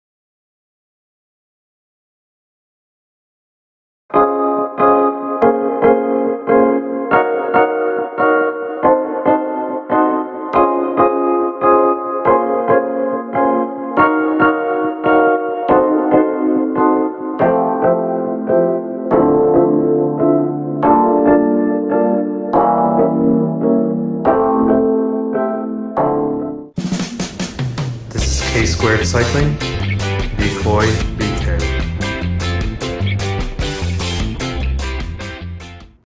MMRx (Movement Medicine) for Cyclists: Lateral Towel Slides
strength and mobility training for cyclist!

KSquared Cycling- be Khoi, be Khang!

my gear:
data management: Garmin 830
front camera: GoPro Session
rear camera/light combo: Cycliq Fly6
power management: Cyclops Powertap G3